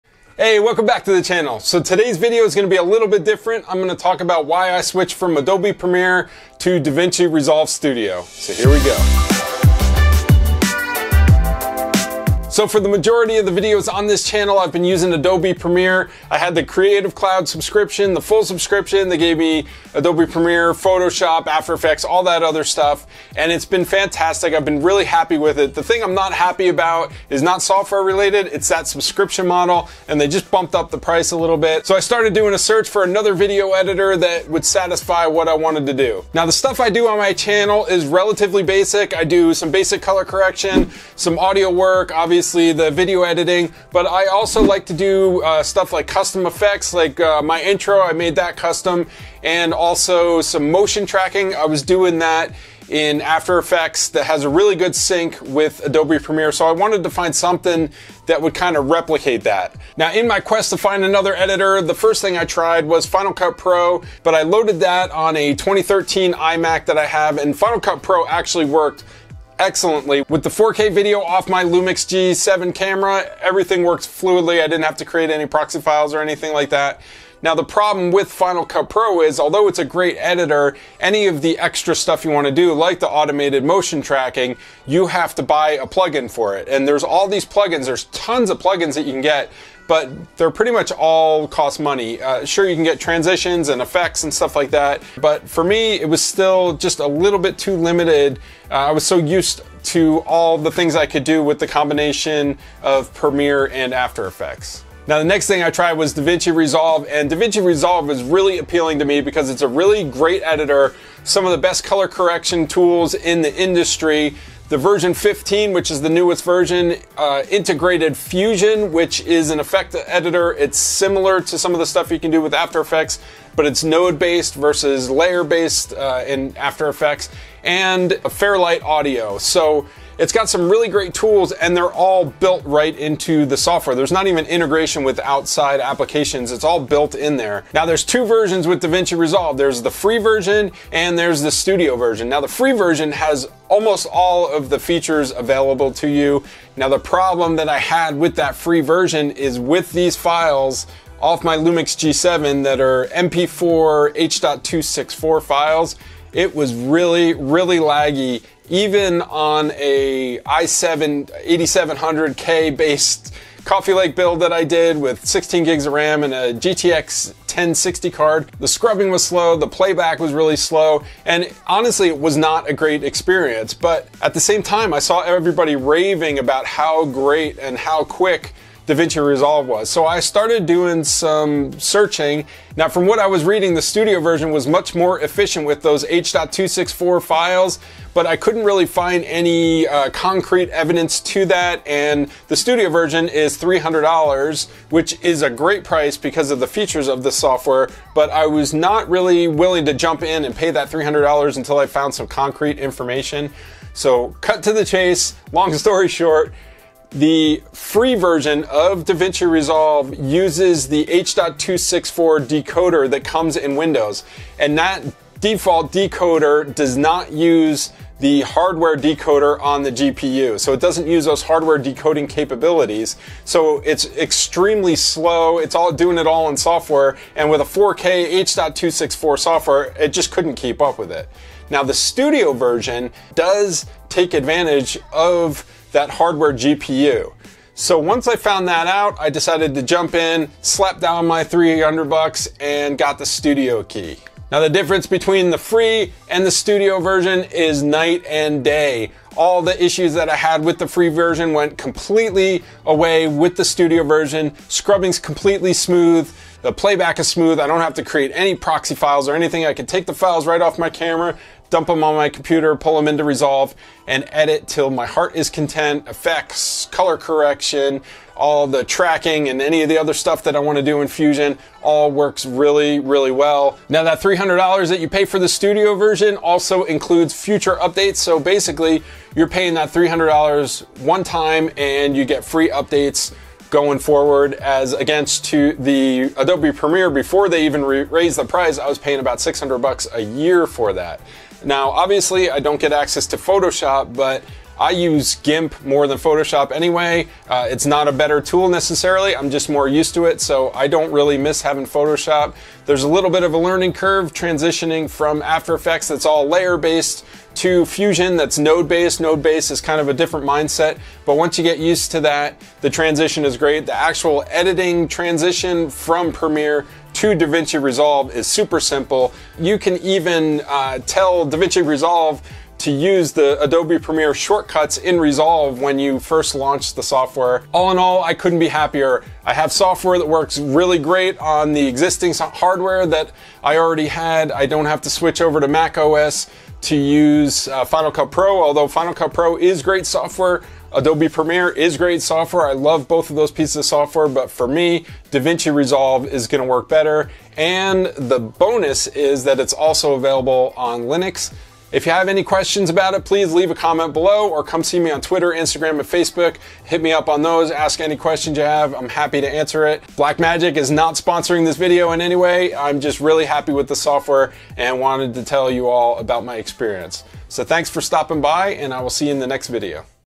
Why I switched from Adobe Premiere to DaVinci Resolve Studio
For those looking to get out from under the subscription model of Adobe Premiere, DaVinci Resolve Studio is a great option and was the right one for me!

☀ Gear used for the video:
Audio-Technica ATR3350iS Mic
Game capture card